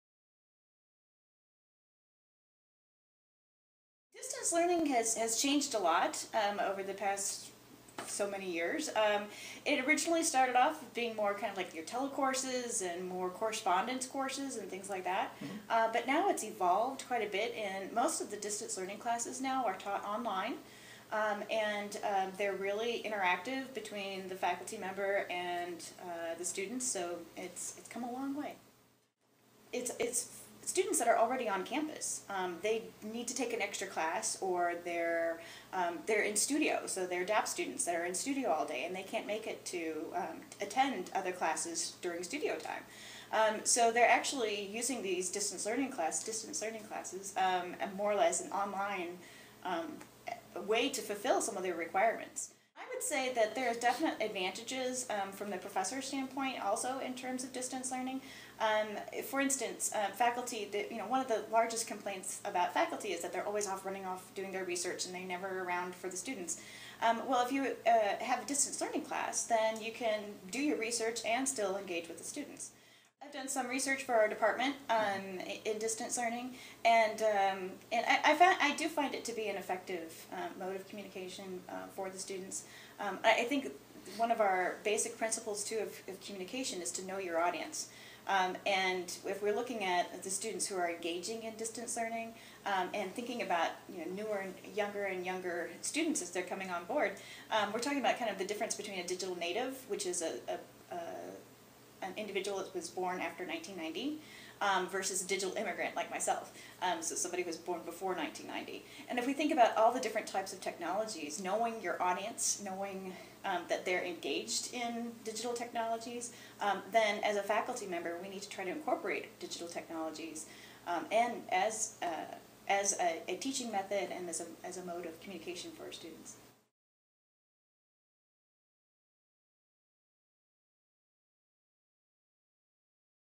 UC Faculty Spotlight Nancy Jennings
Professor Nancy Jennings explains Distance Learning and its impact on the educational landscape.